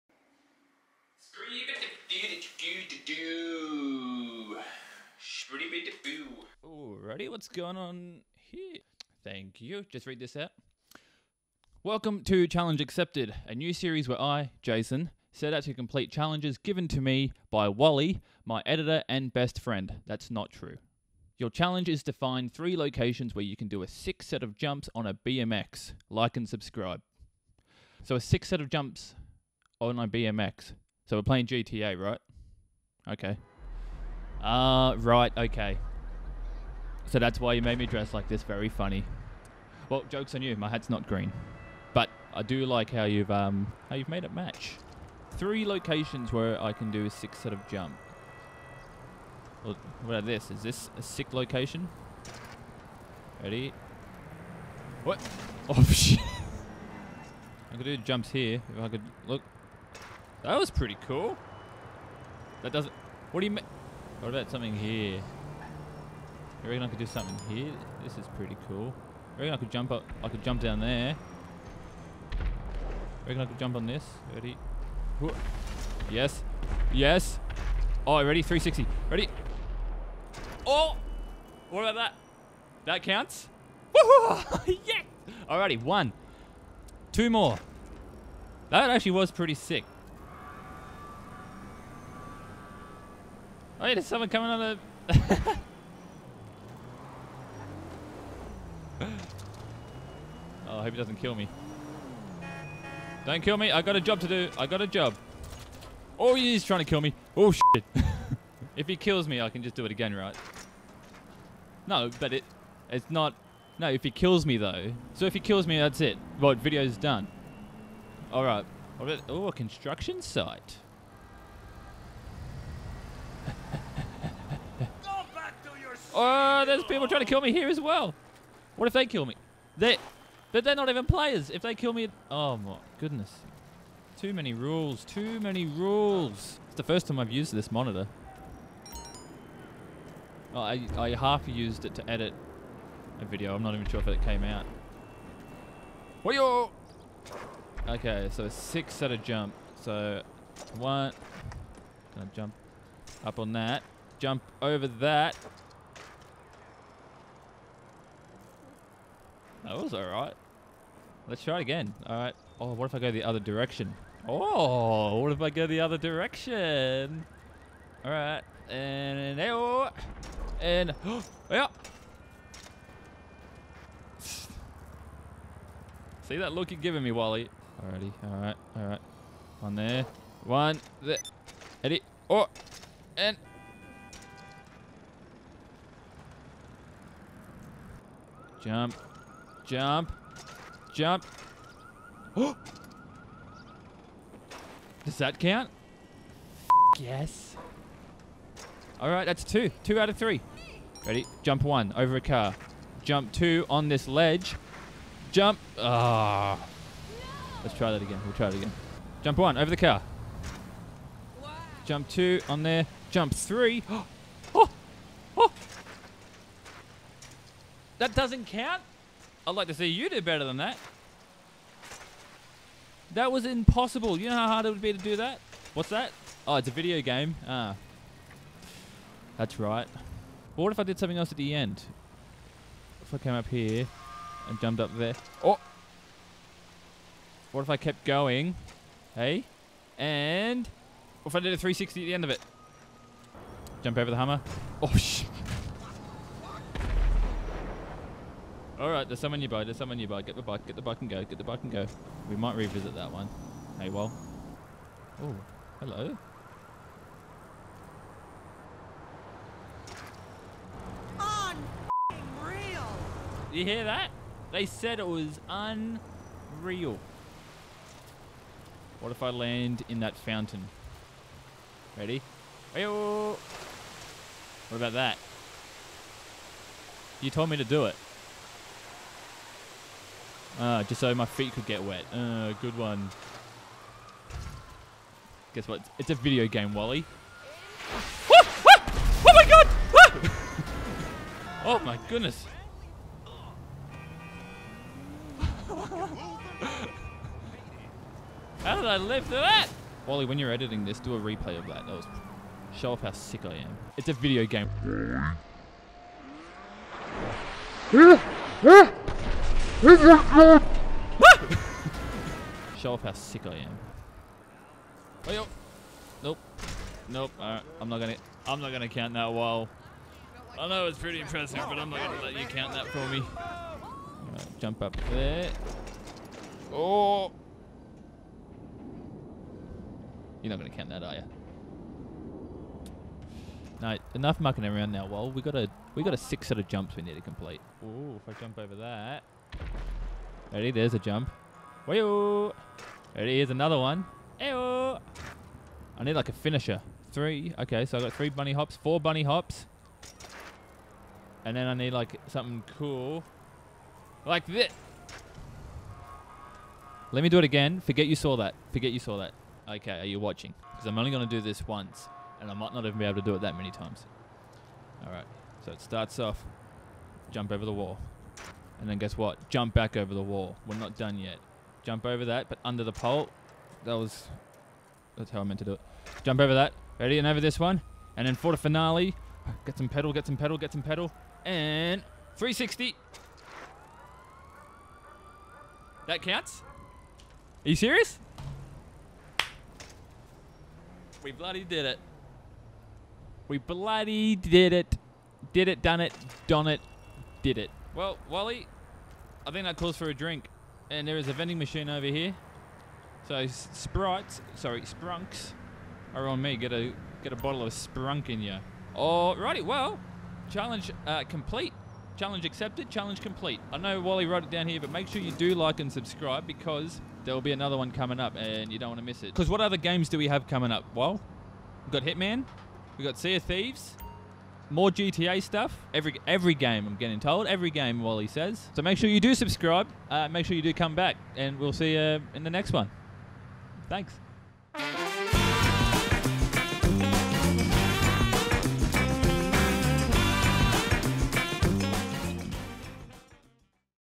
Find 3 Fully Sick BMX Locations | Challenge Accepted
Welcome to a new series called Challenge Accepted, where I am set a challenge to complete either in a videogame or real life. This is not a gaming channel. I repeat; this is not a gaming channel!